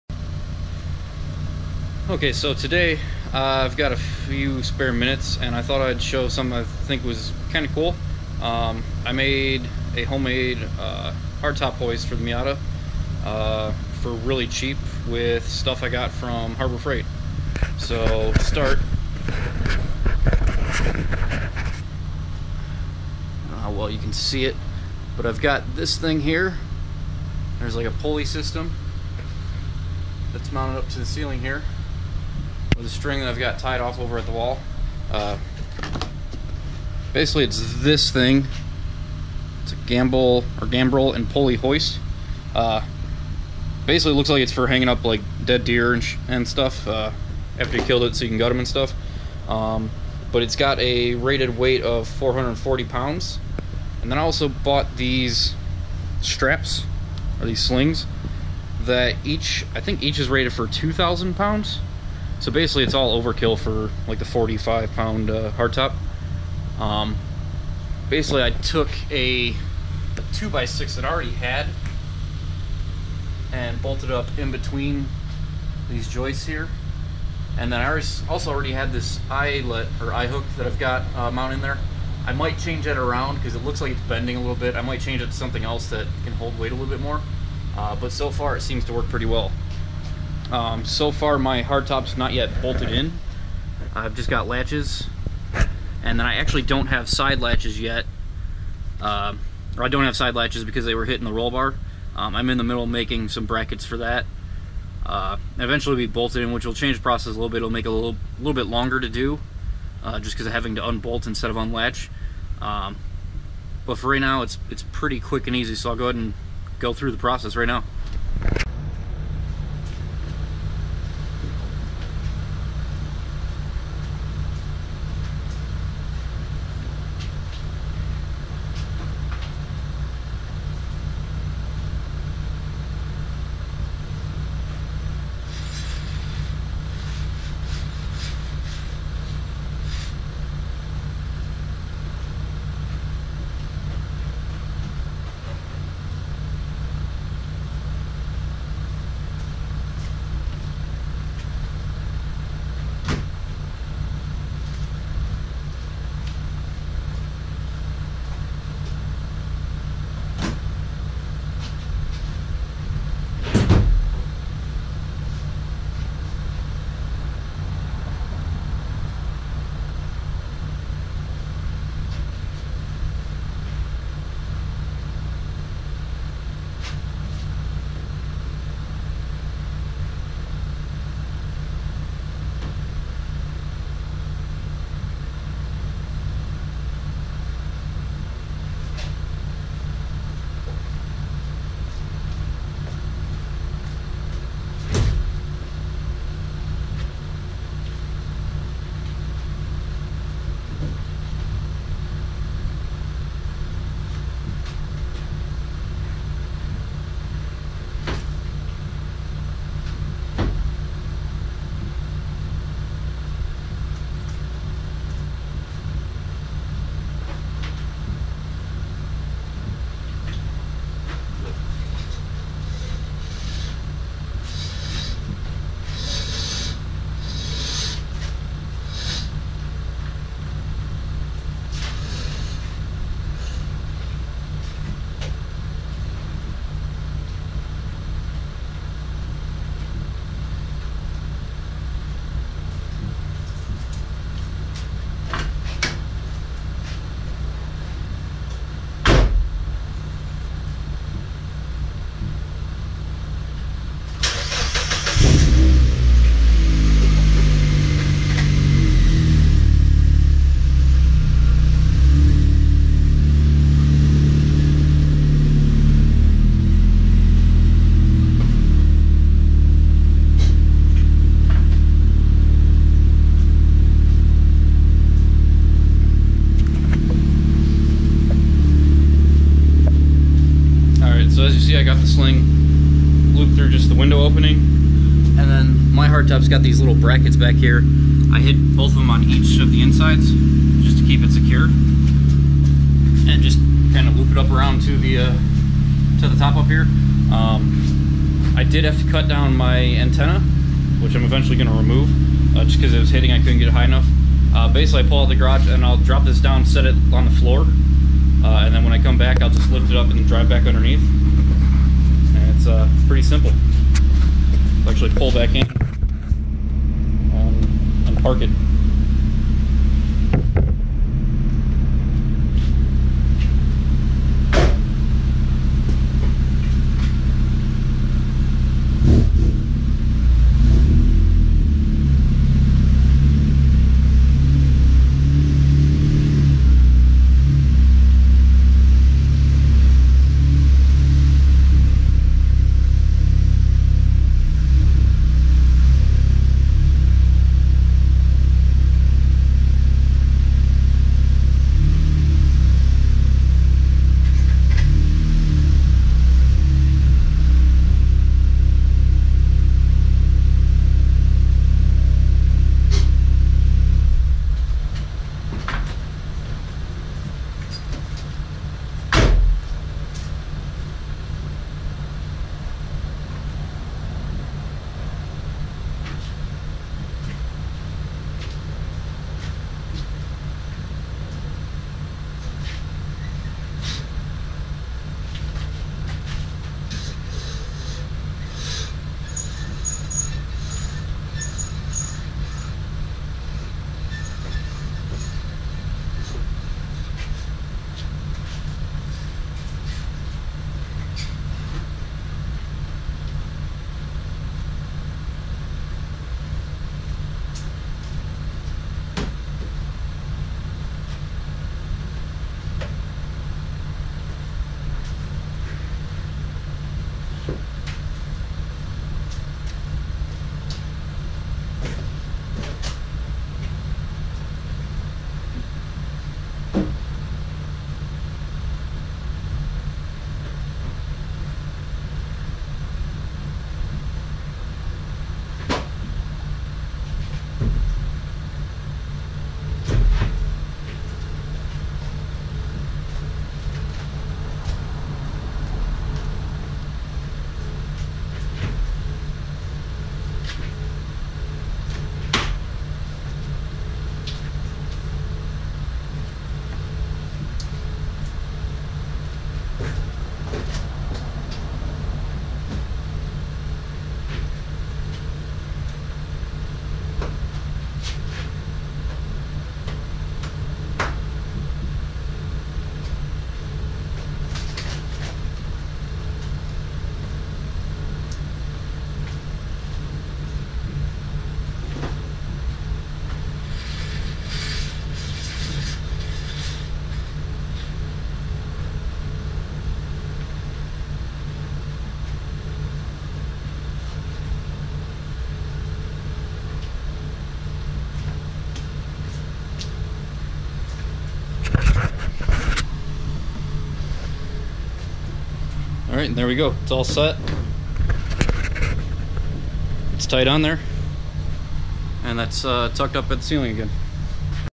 Hardtop Hoist
Homemade hardtop hoist using $30 worth of stuff from everyone's favorite cheap tool store.